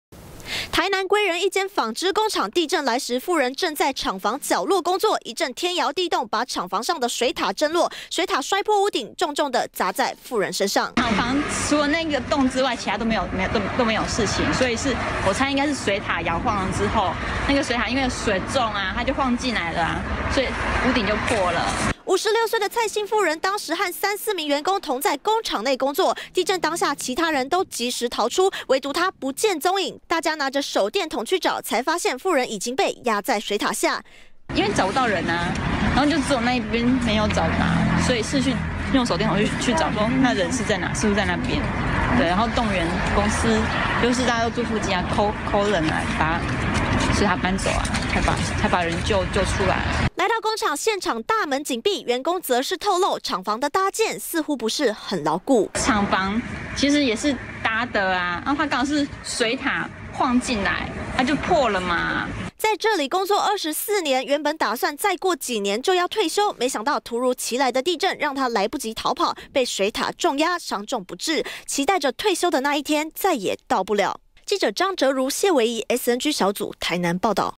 20160206中天新聞 水塔遇震壓破屋頂 砸死產線女員工
台南歸仁這間紡織工廠，水塔不敵強震，摔破屋頂，直接壓倒當時在廠房工作的56歲蔡姓婦人，傷重不治。